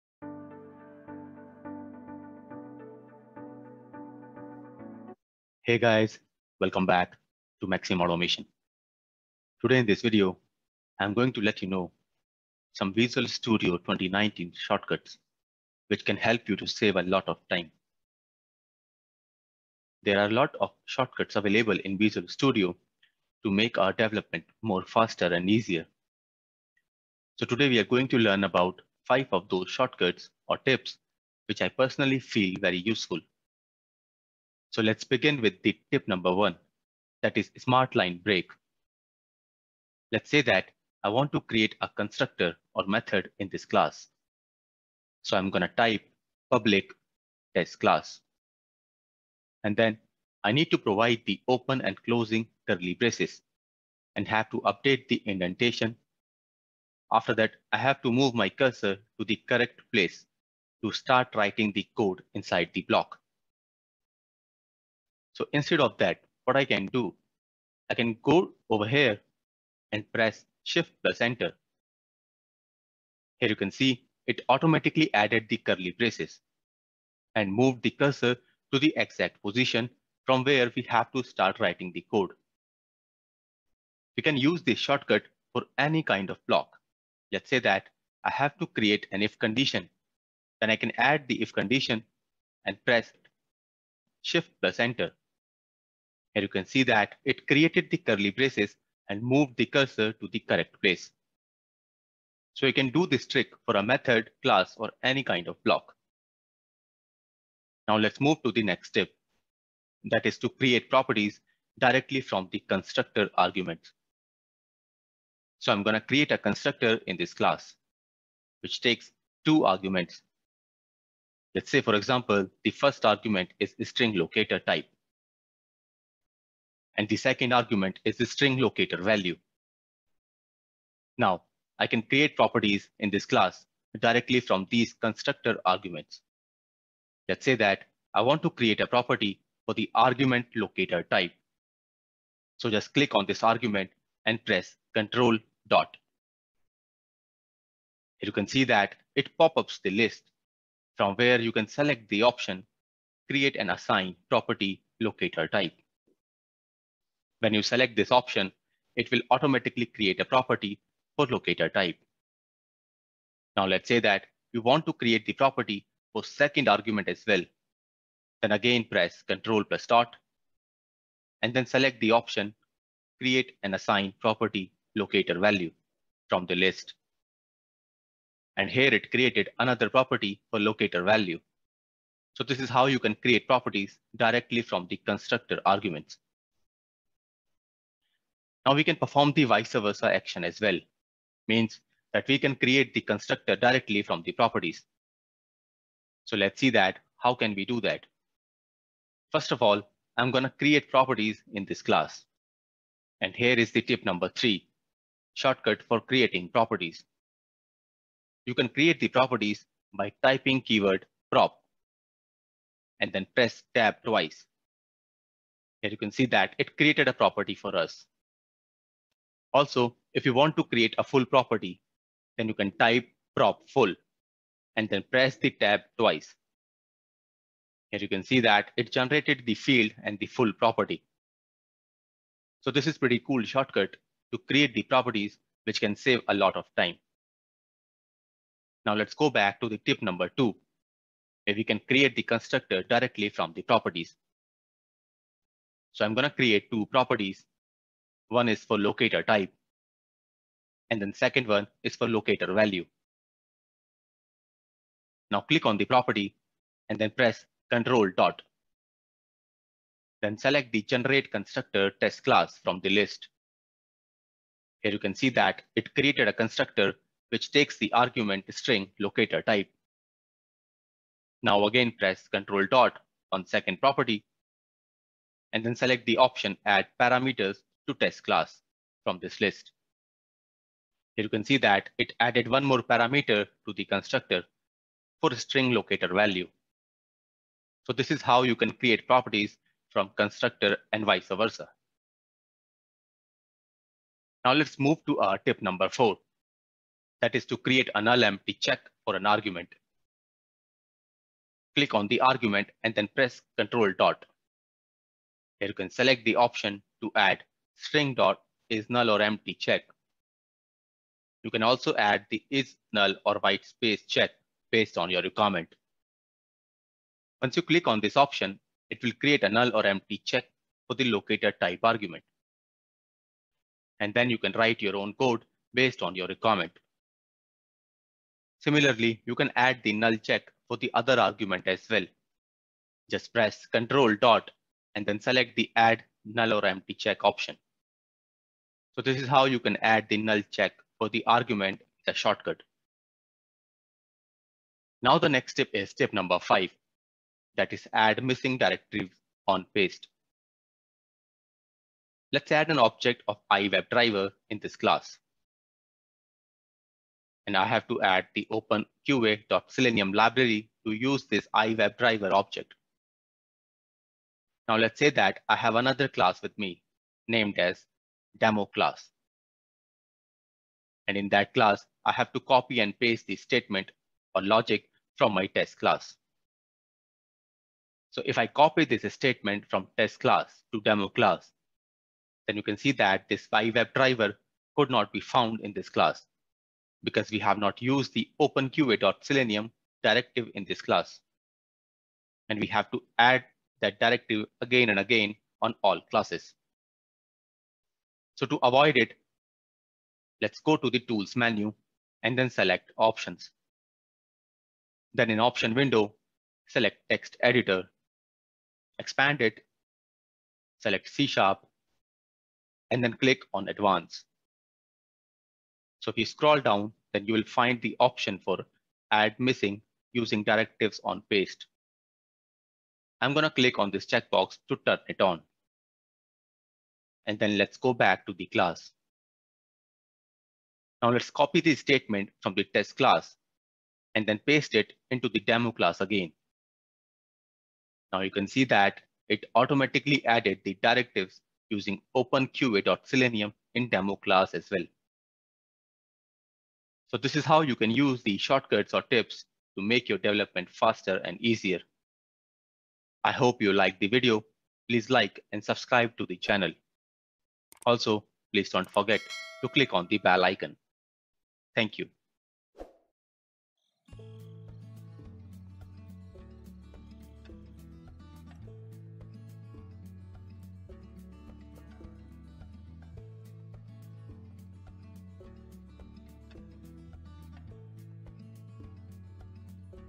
Visual Studio Productivity Hacks You Need to Know | Must-Know Productivity Tips for Visual Studio
Want to code faster and smarter? In this video, discover essential productivity hacks and tips for Visual Studio that every developer should know. Whether you're a beginner or a seasoned pro, these tricks will help you streamline your workflow and boost your coding efficiency.

🔍 What You’ll Learn:
Time-saving keyboard shortcuts

Efficient code navigation techniques

Powerful debugging tips and tricks

Customizing Visual Studio for optimal productivity

Useful extensions and tools to enhance your development experience

Best practices to speed up your day-to-day coding tasks

🧰 Tools & Features Covered:
Visual Studio IDE (all recent versions)

Built-in productivity tools and extensions

💬 Have your own favorite tips? Share them in the comments below!

Please comment in case of any query

Please watch below video as well -